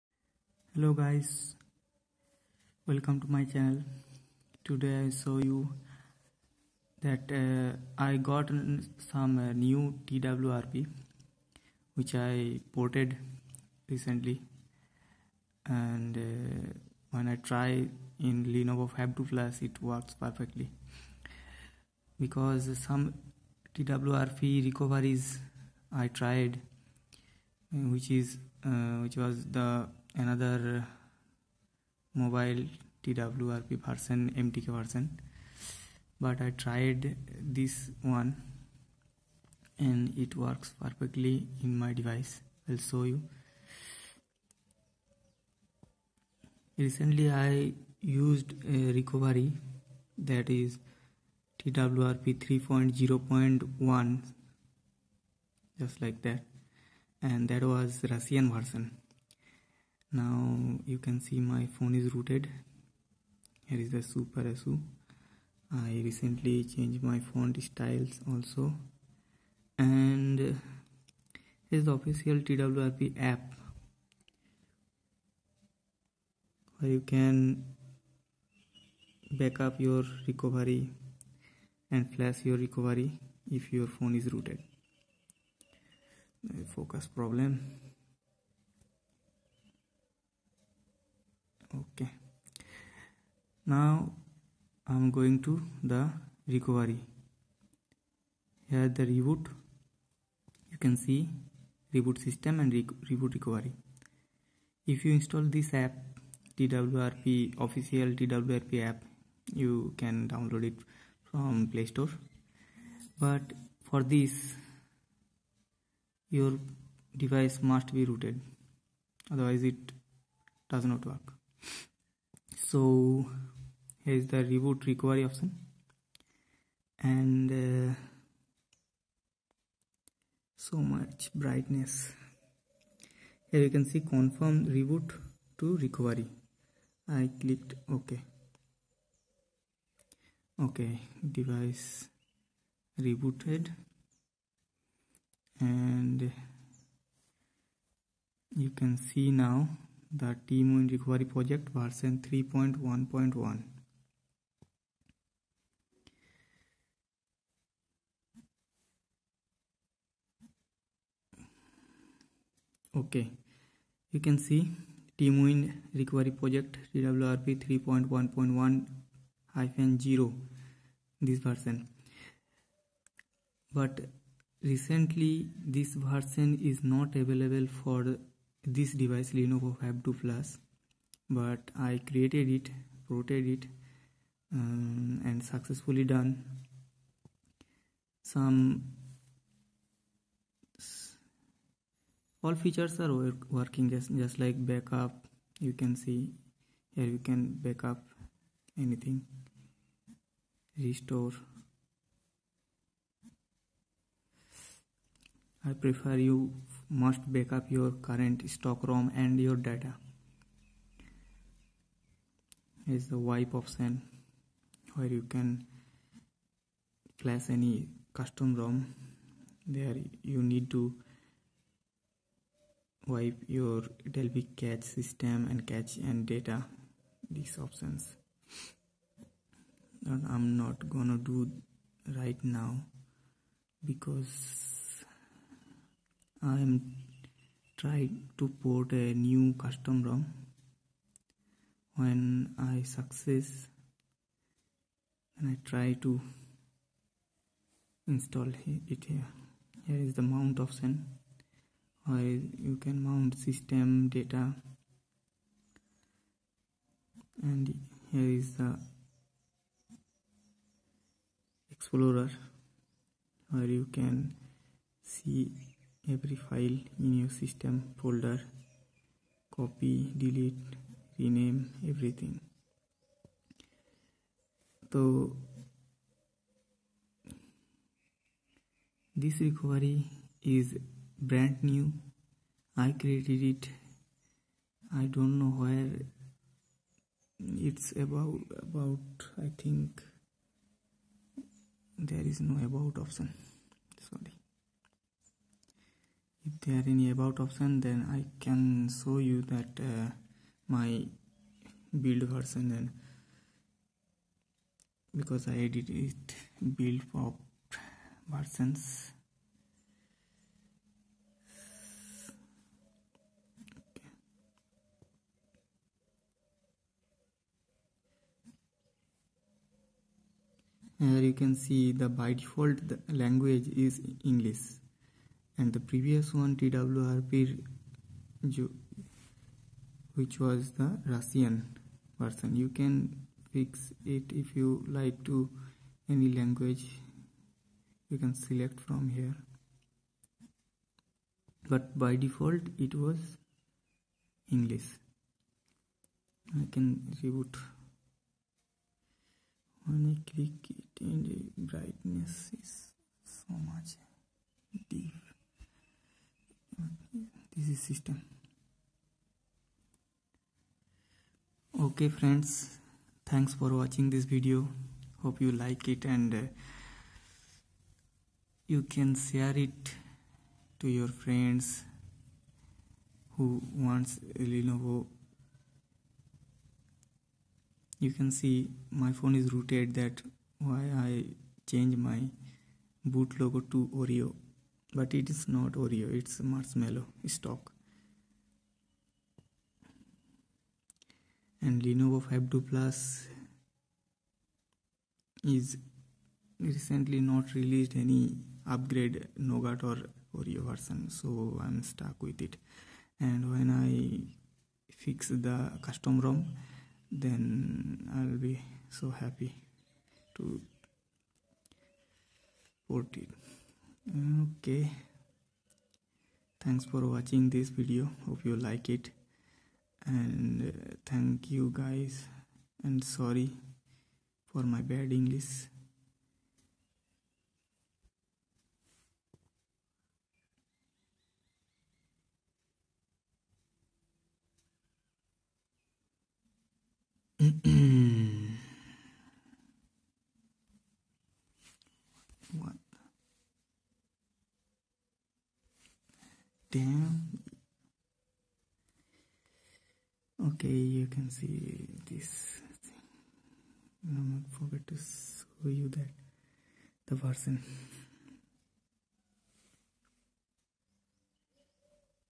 New TWRP 3.1.1-0 recovery for Lenovo phab 2 plus (PB2-670M)
I ported this twrp recovery 3.1.1 for Lenovo phab2 plus today 08/12/17

For recovery.img check my new website for update.